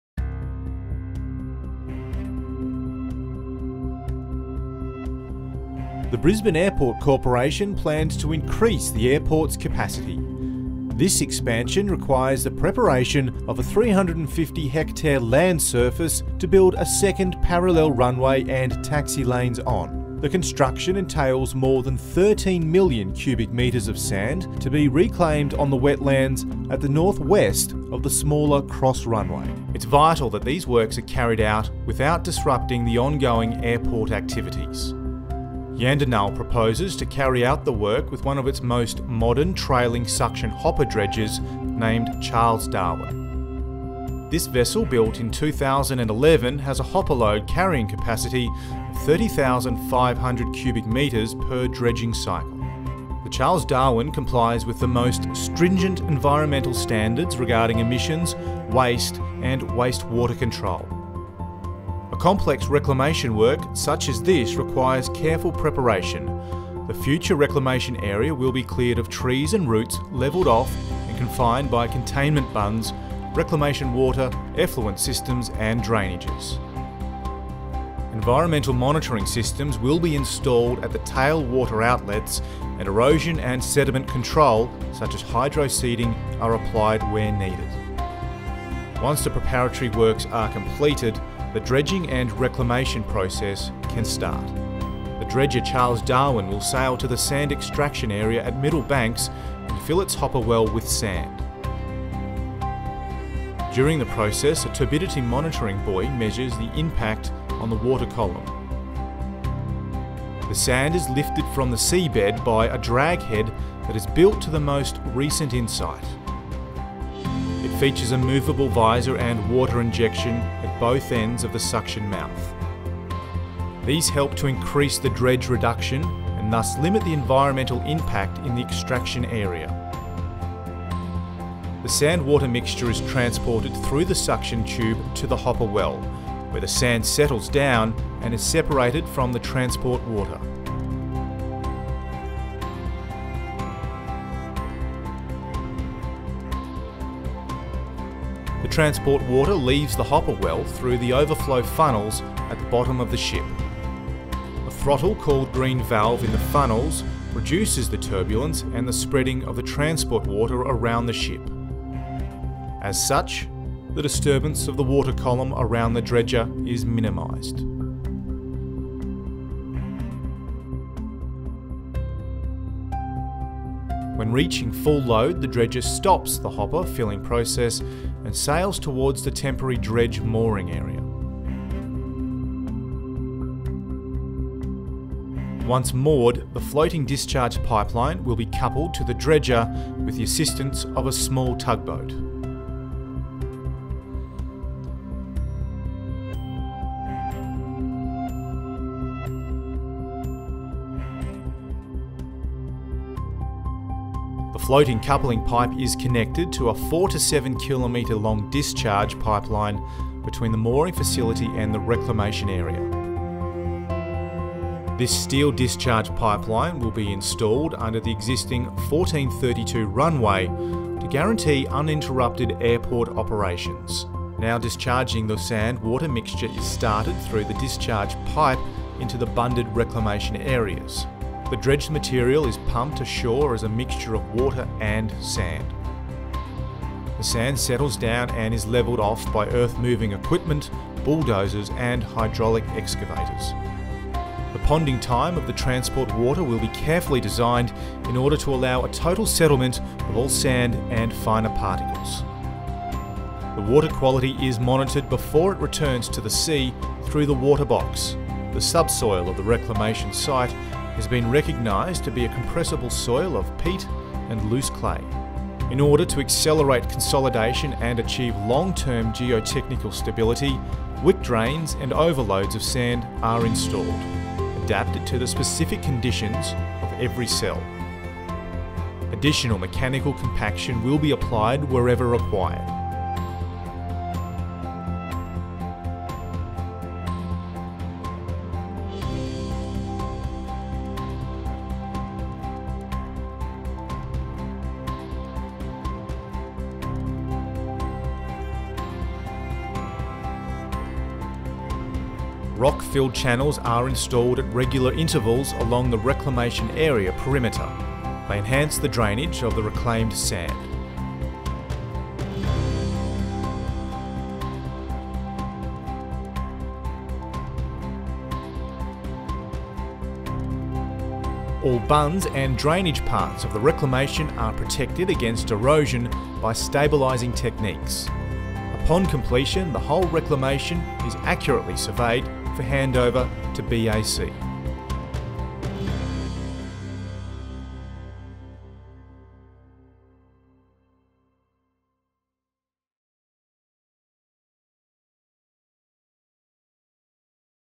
Brisbane's new parallel runway project - Charles Darwin Jan de Nul's Dredge & Reclamation works
This video explains the complex reclamation and dredging work undertaken by Jan de Nul for Brisbane Airport's new parallel runway site.

Brisbane’s new runway is the completion of long-term planning for Brisbane Airport which commenced in the 1970s. Construction of the new runway began in 2012, with the official opening on Sunday 12 July 2020.

The new runway gives Brisbane the most efficient runway system in Australia. It will serve the needs of South East Queensland for decades to come, maintaining and growing our connections with the world as our city continues to grow, while building the gateway to Queensland & Australia.